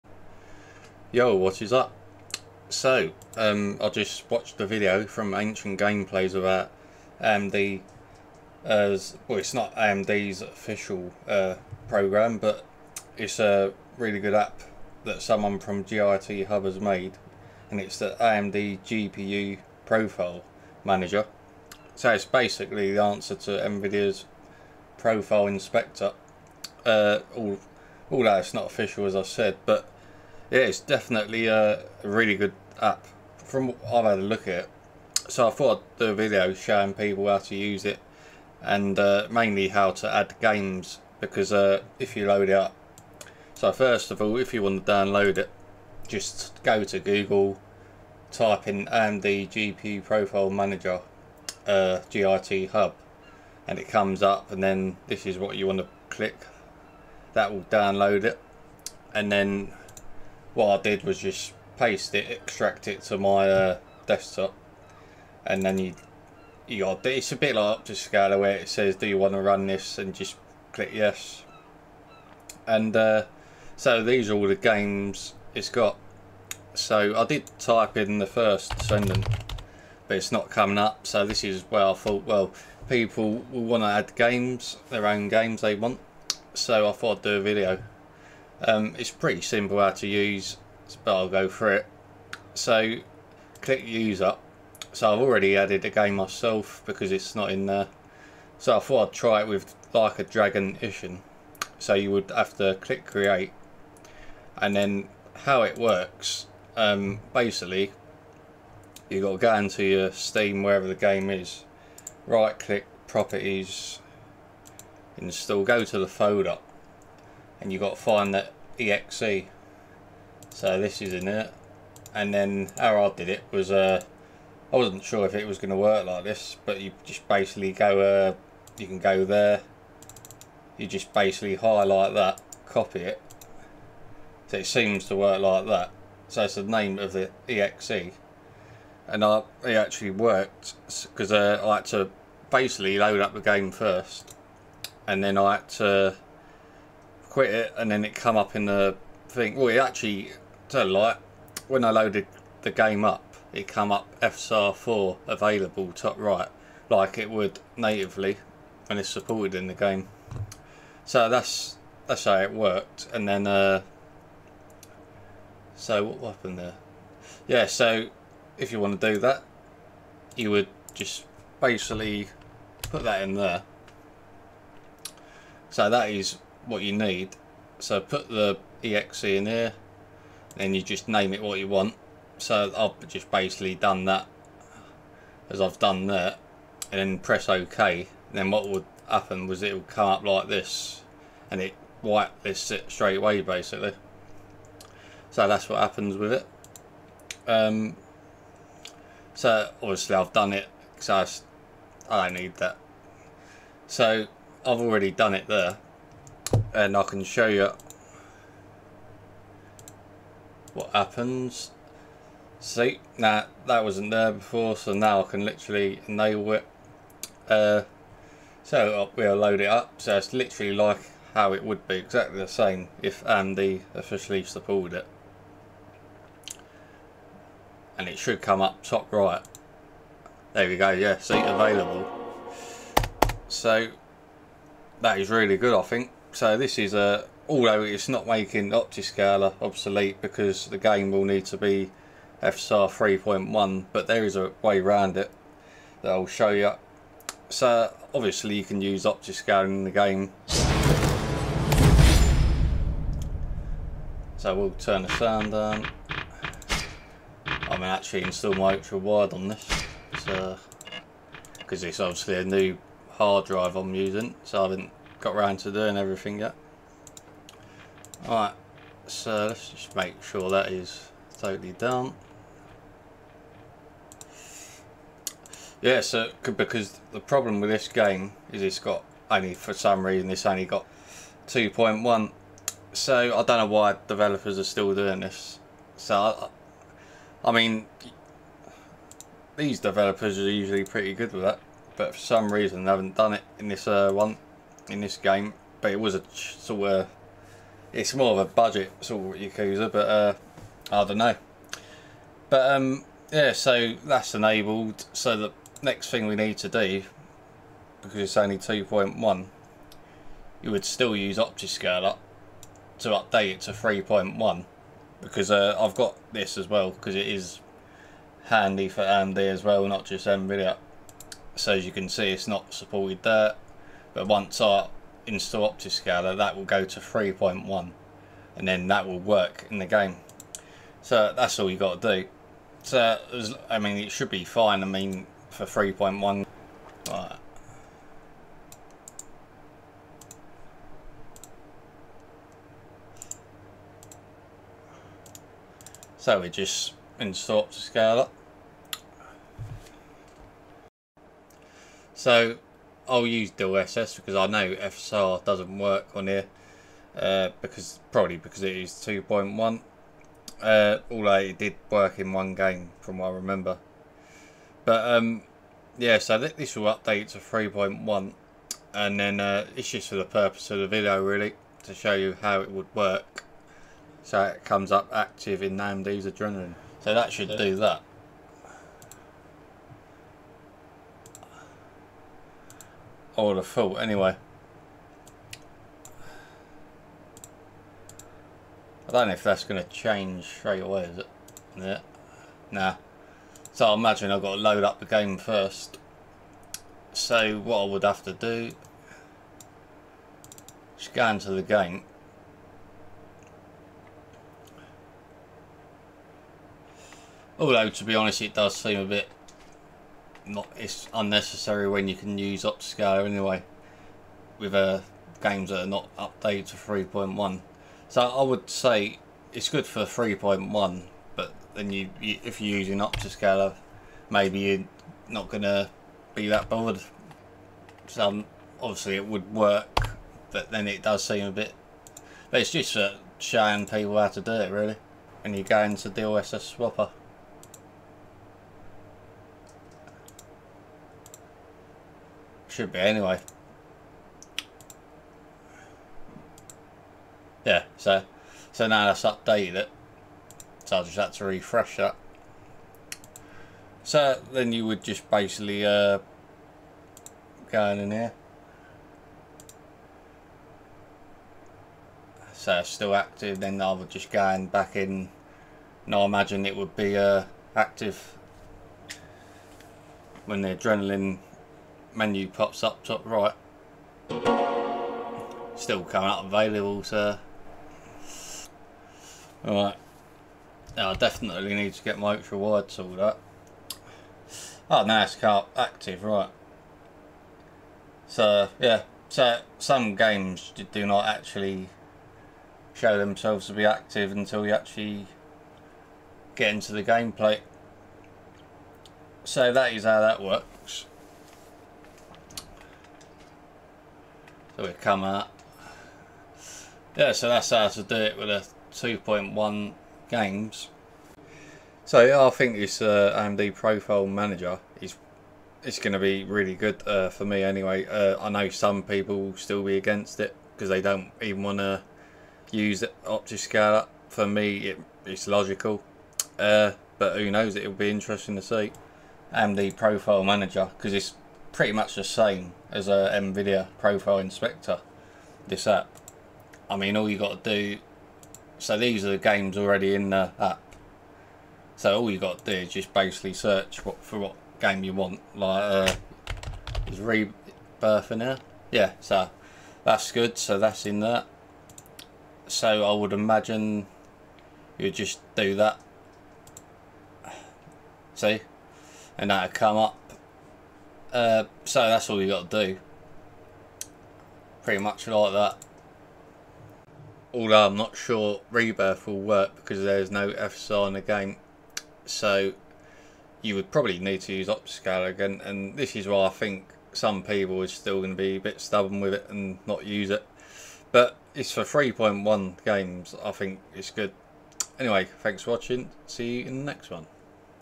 AMD GPU Profile Manager! -Add FSR 4 Adrenalin support yourself! How to use & also create profiles!
Great App from Github which is similar to Nvidias Profile Inspector! Although it's a very early version but it works!

I think it mainly makes sense for Games that already support FSR 3.1 otherwise i'd use Optiscaler